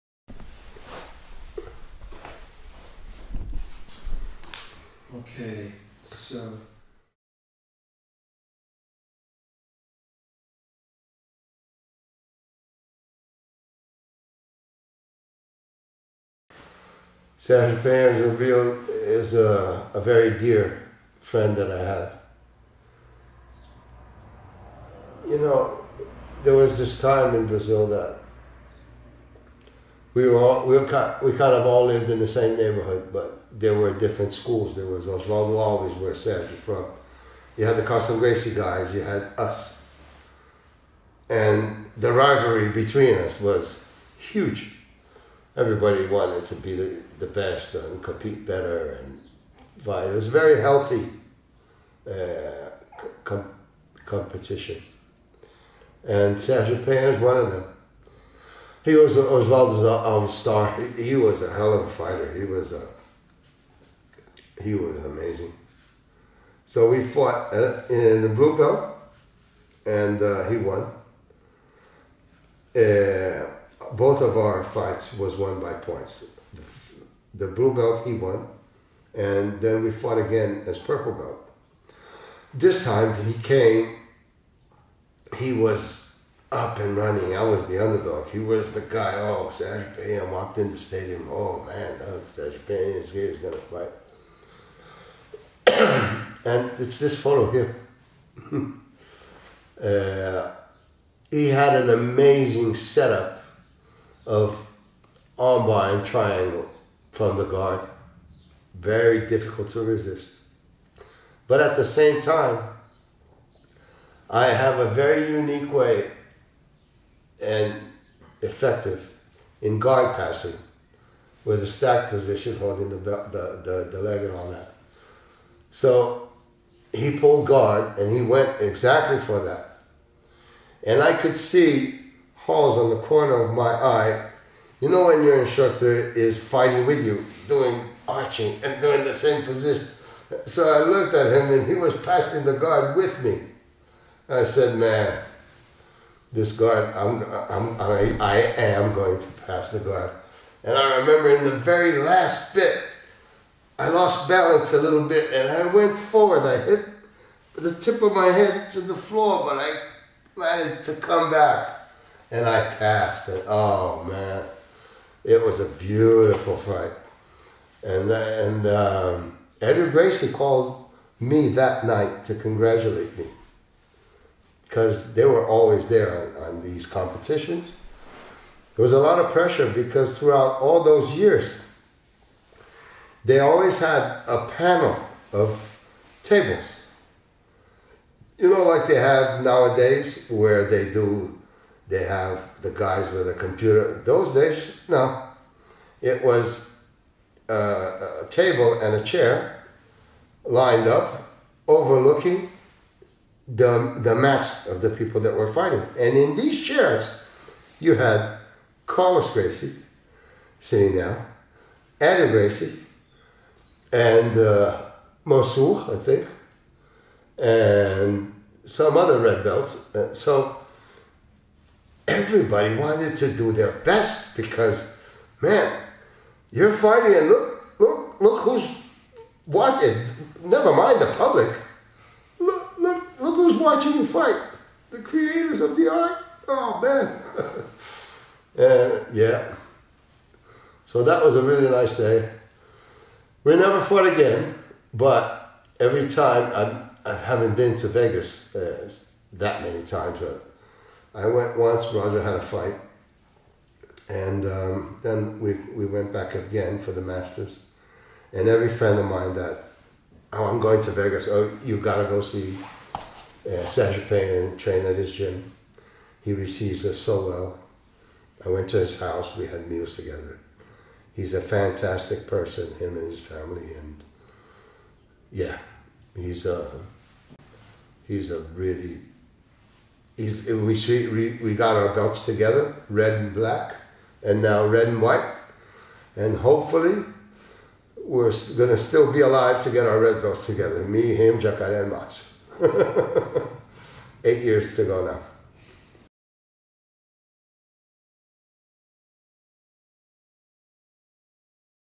What are Mauricio's Memories of Sergio Penha?
One month after Vanessa was born, Mauricio fought Sergio Penha and lost  They fought again at purple belt, and that time Mauricio won. They remain close friends to this day. We asked Mauricio about his memories of Sergio.

Follow Mauricio on Social Media!